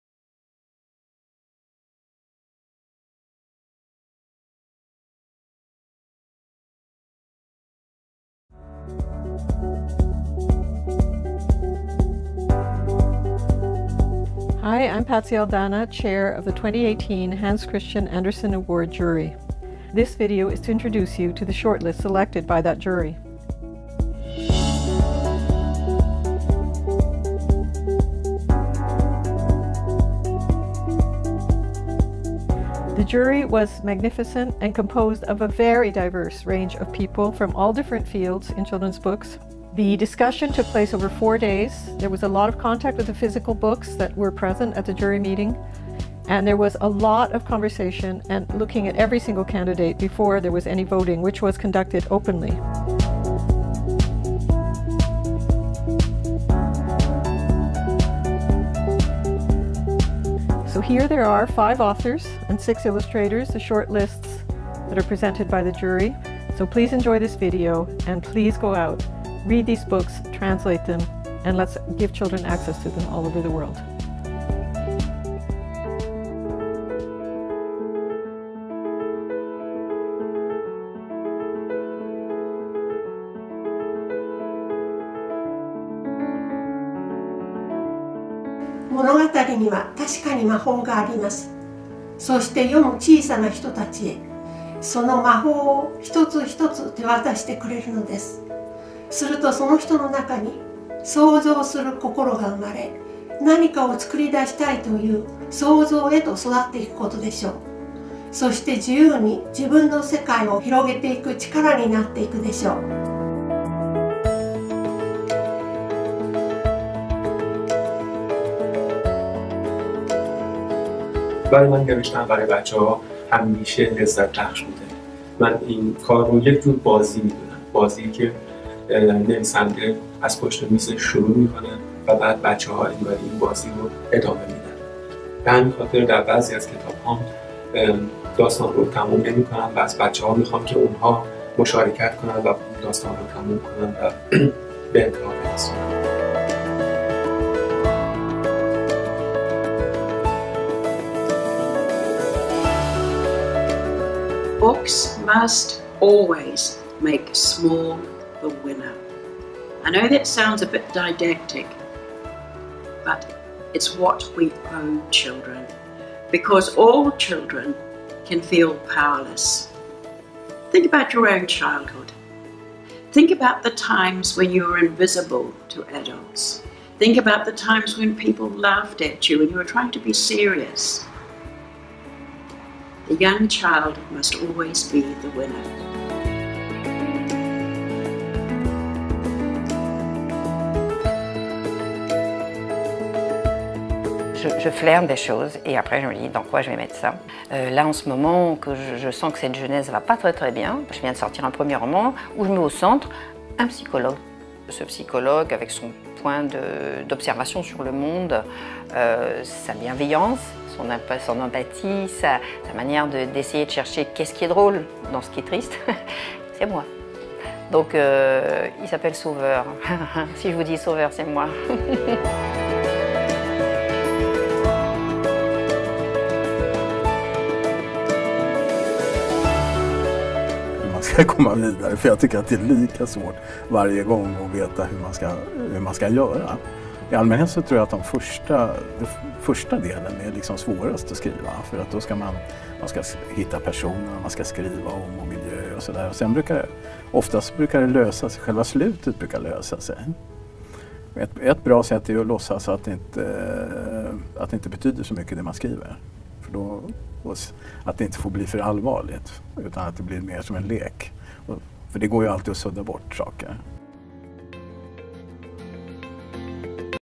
ANDERSEN AWARD 2018 SHORTLIST 2
Final nominees for the authors of the final list of the Hans Christian Andersen Award of the Year 2018